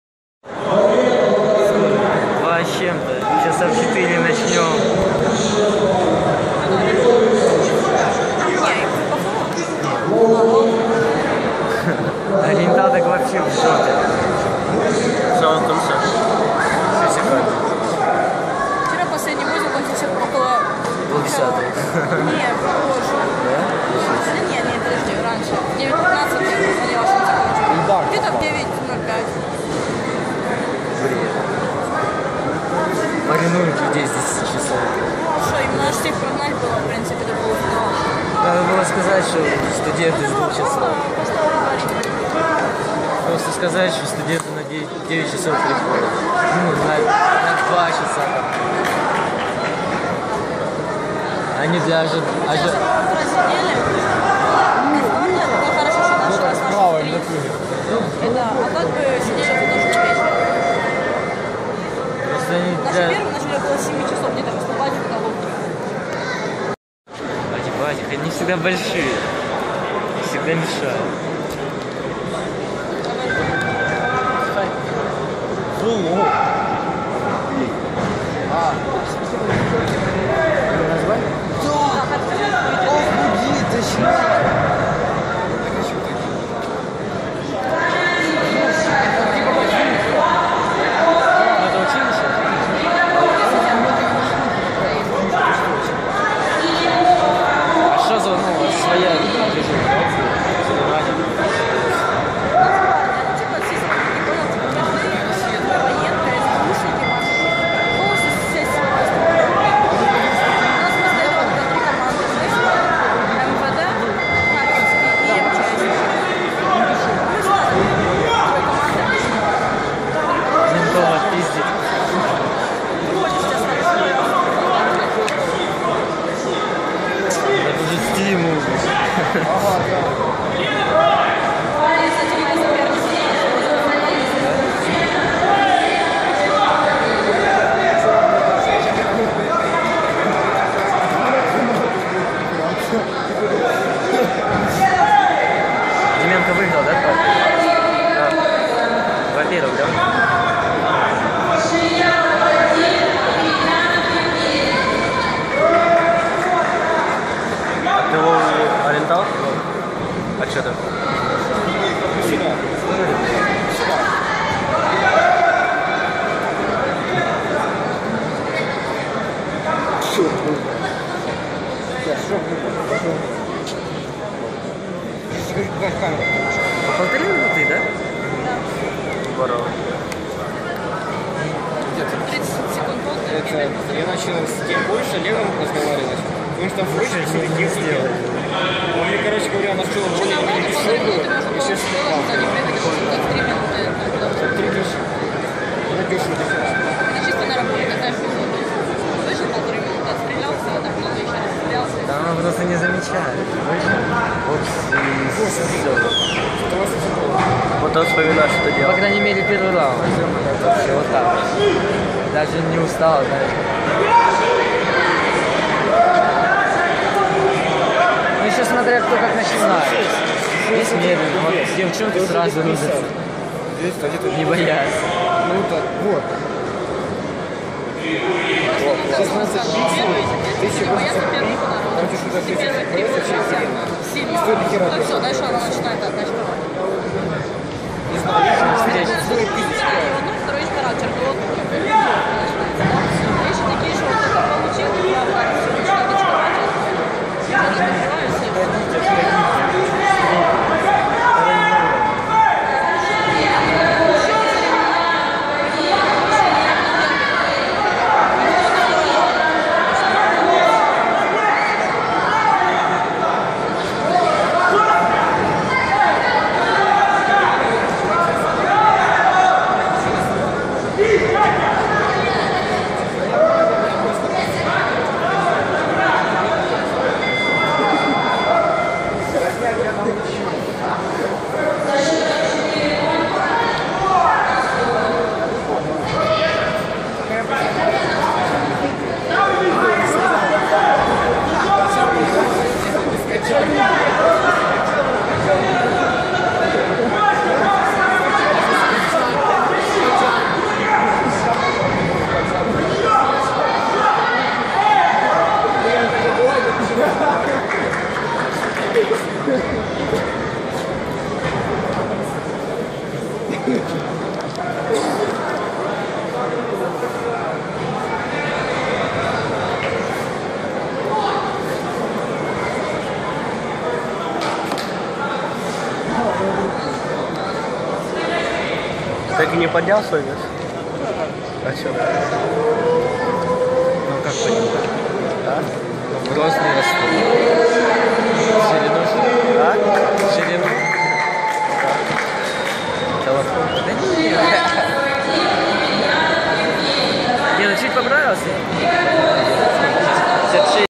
Мельник Наташа финал ОРИЕНТАЛ
Чемпионат Украины среди юниоров по кикбоксингу WPKA 2-5 апреля 2015